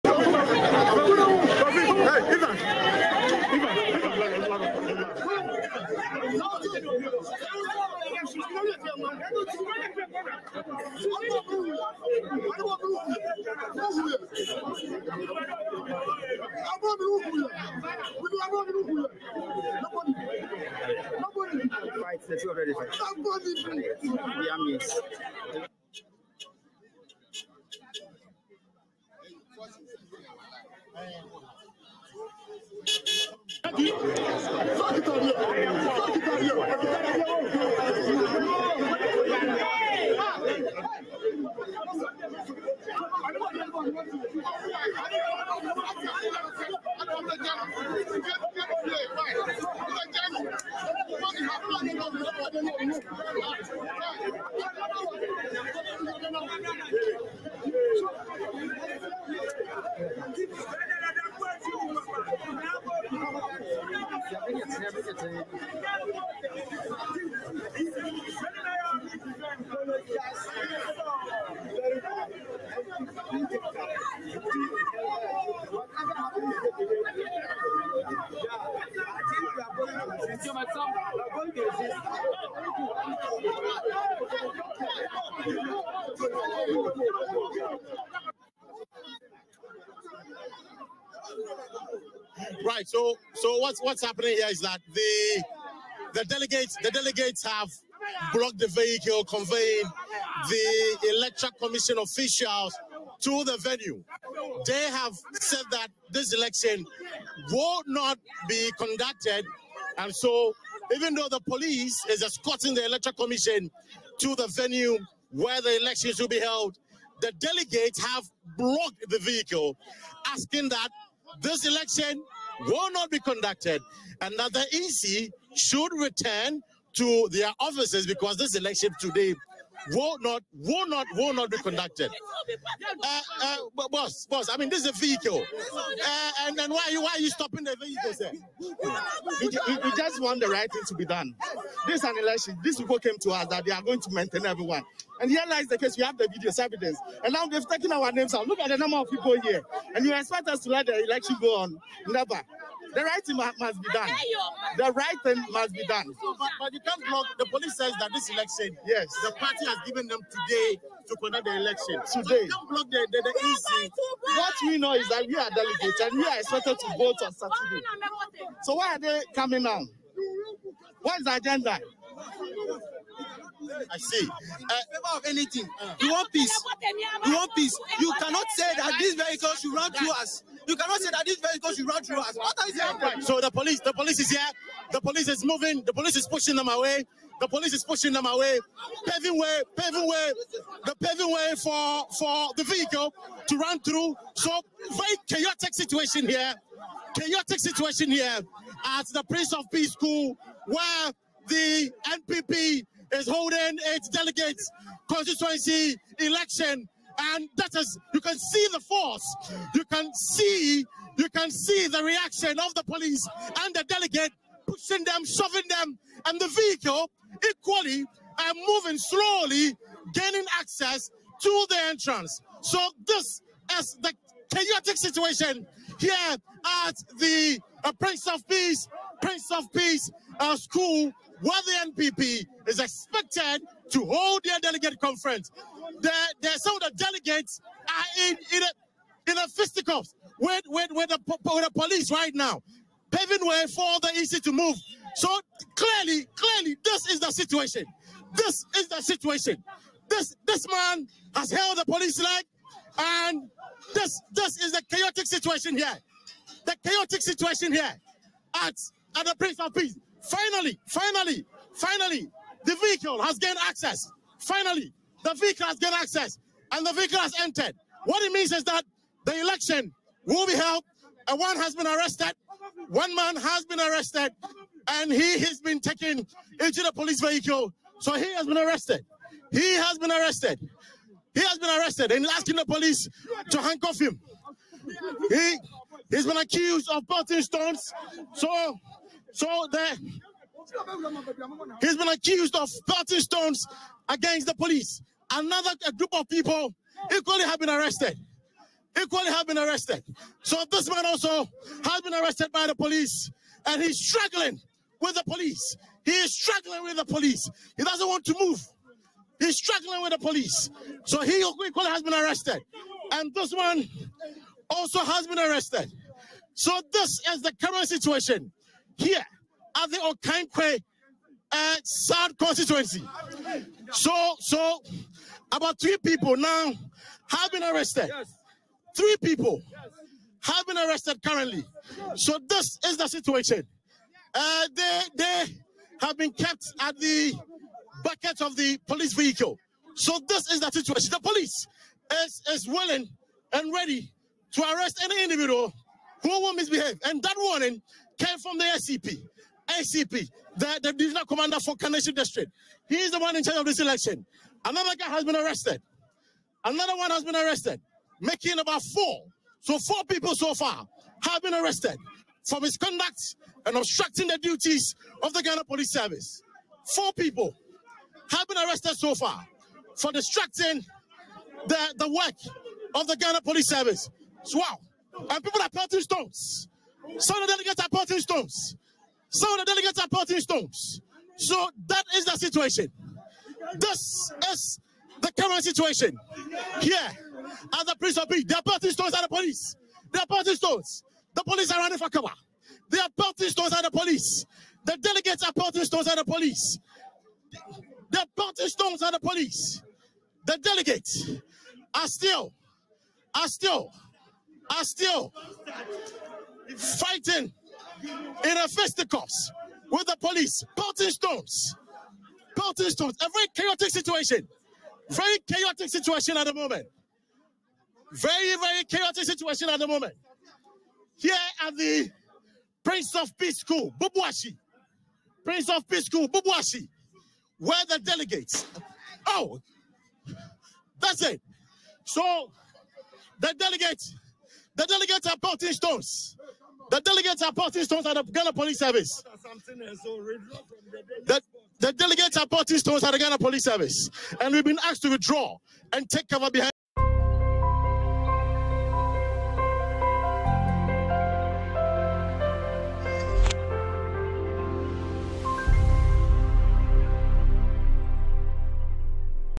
NPP Constituency Elections: Some delegates at Okaikwei South clash with police over conduct of polls
Four delegates of the New Patriotic Party (NPP) have been arrested for obstructing the party’s constituency elections in Okaikwei South.